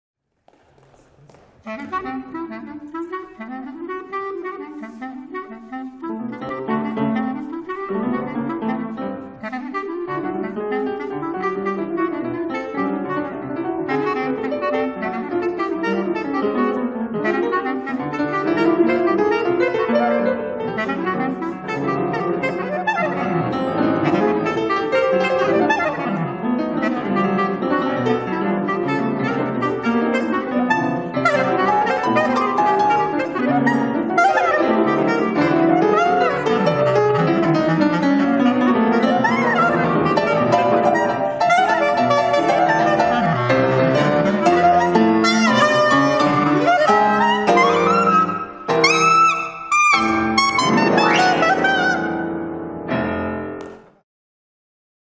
Köppen Jörgensmann reunited, live at Neue Kirche Wuppertal 2013 - 09
Piano: Bernd Köppen
Clarinet: Theo Jörgensmann

Recorded live at Neue Kirche Wuppertal, 2013
Video by Markus van Elsen